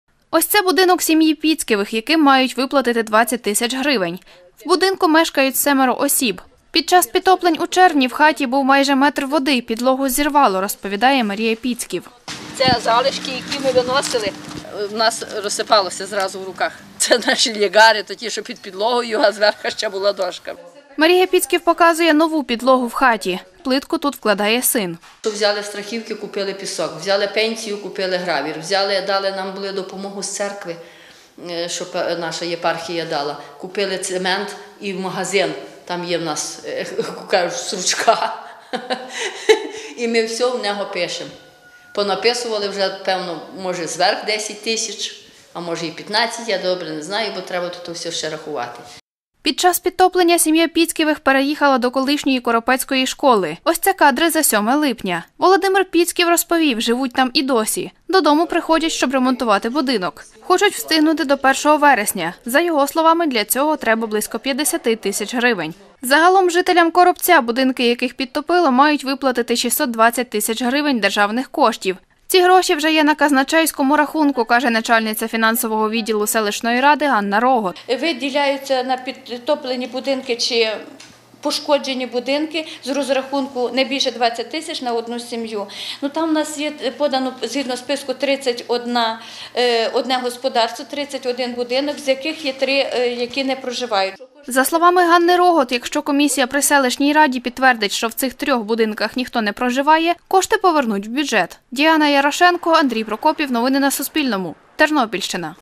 На Тернопільщині постраждалим від паводку виплатять компенсацію
Сім’я Піцьківих з Коропця другий місяць живе в приміщенні колишньої школи. Їхній будинок підтопило через паводок на Дністрі 25-го червня. Грошей, щоб відремонтувати житло люди не мають, – розповів Володимир Піцьків